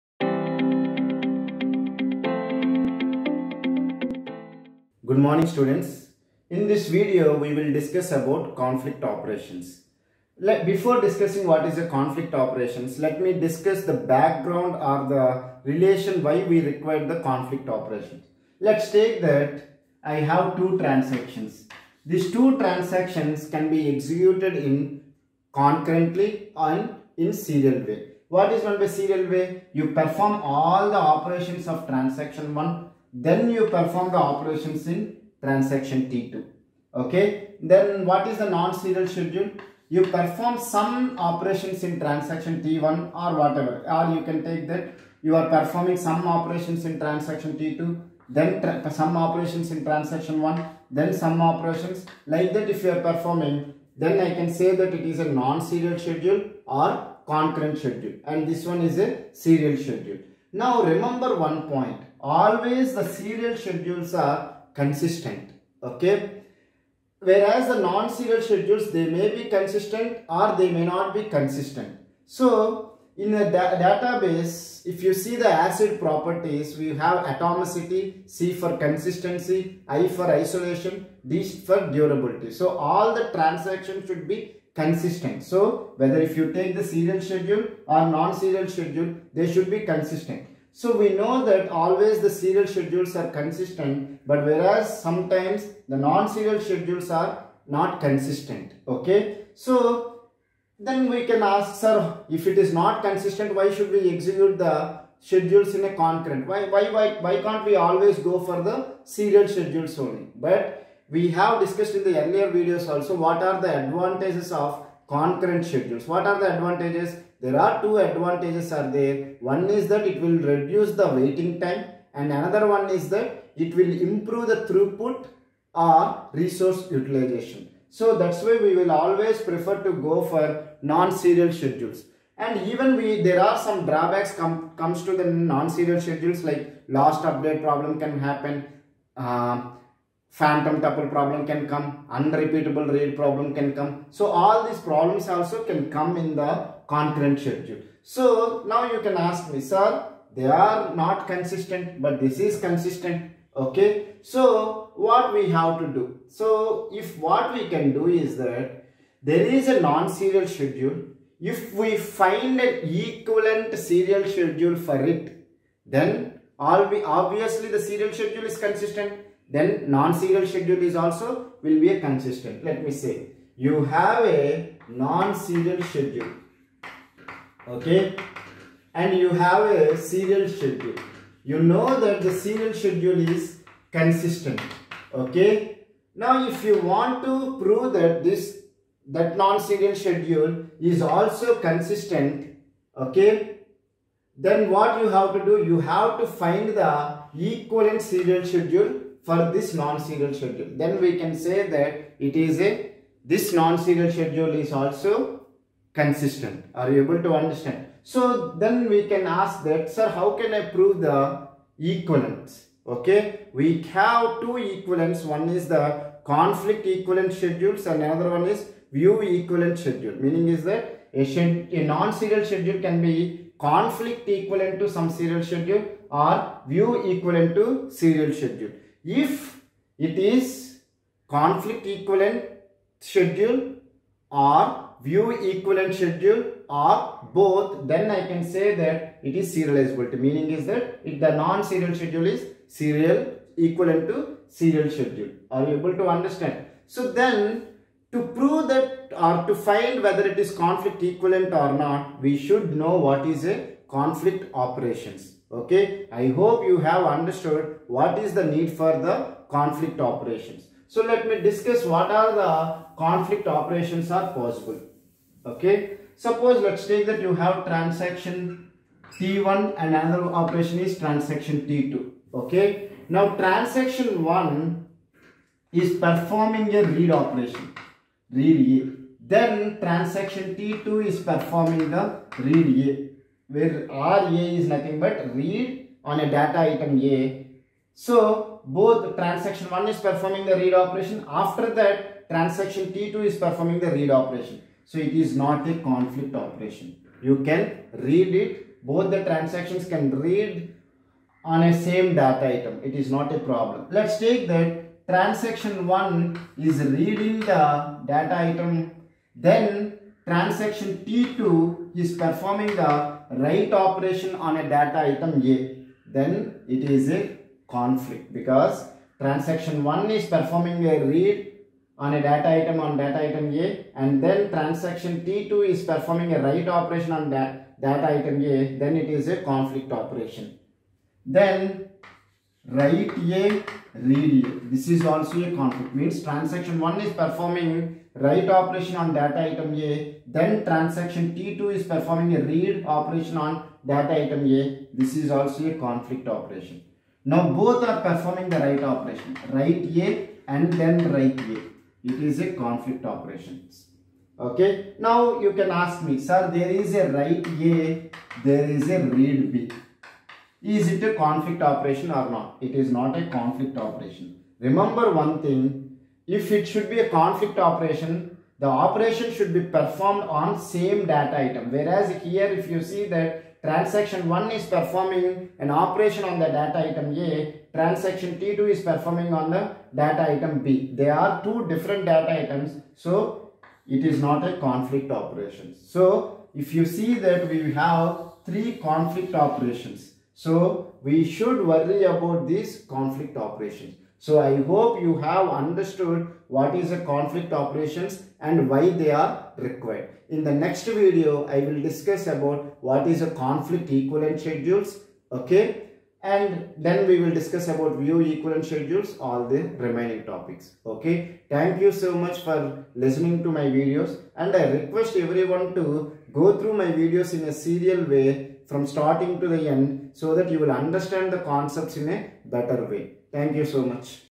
L 70: CONFLICT OPERATIONS | DBMS LECTURES | GATE CSE LECTURES | DBMS Full Course
In this video, i have discussed what are the conflict operations possible.